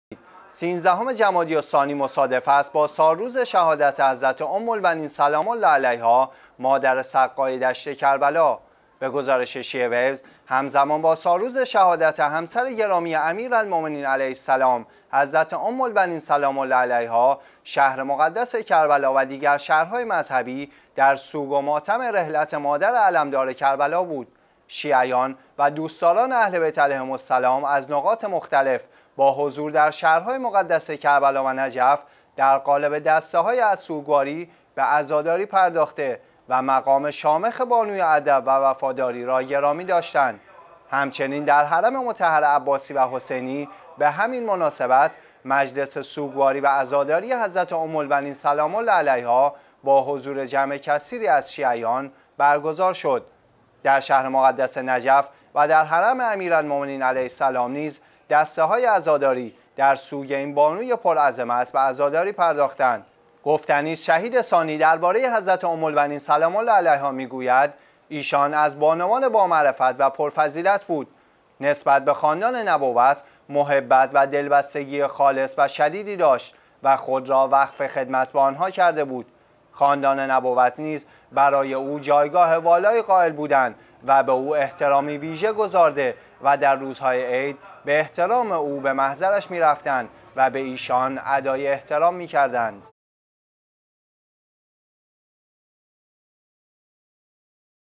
سيزدهم جمادى الثانى شهادت حضرت ام البنین سلام الله علیها
سيزدهم جمادی الثاني مصادف است با سالروز شهادت حضرت ام البنين سلام الله عليها مادر سقای دشت کربلا.
به گزارش شیعه ویوز، همزمان با سالروز شهادت همسر گرامی امیرالمومنین علیه السلام، «حضرت ام البنین سلام الله علیها» شهر مقدس کربلا و دیگر شهرهای مذهبی در سوگ و ماتم رحلت مادر علمدار کربلا بود.
شیعیان و دوست داران اهل بیت علیهم السلام از نقاط مختلف، با حضور در شهرهاى مقدس کربلا و نجف در قالب دسته های سوگواری به عزاداری پرداخته و مقام شامخ بانوی ادب و وفاداری را گرامی داشتند.
همچنین در حرم مطهر عباسی و حسینی به همین مناسبت مجلس سوگواری و عزاداری حضرت ام البنین سلام الله علیها، با حضور جمع کثیری از شیعیان برگزار شد.
 در شهر مقدس نجف و در حرم اميرالمومنين علیه السلام نیز دسته هاى عزادارى در سوگ اين بانوى پر عظمت به عزادارى پرداختند.
گفتنی است «شهید ثانی» درباره حضرت ام ‏البنین سلام الله علیها می‌‏گوید: «ايشان از بانوان با معرفت و پر فضیلت بود. نسبت به خاندان نبوت، محبت و دلبستگی خالص و شدیدی داشت و خود را وقف خدمت به آن‏ها کرده بود. خاندان نبوت نیز برای او جایگاه والایی قائل بودند و به او احترامی ویژه گذارده و در روزهای عید، به احترام او به محضرش می‌رفتند و به ایشان ادای احترام می‌کردند.»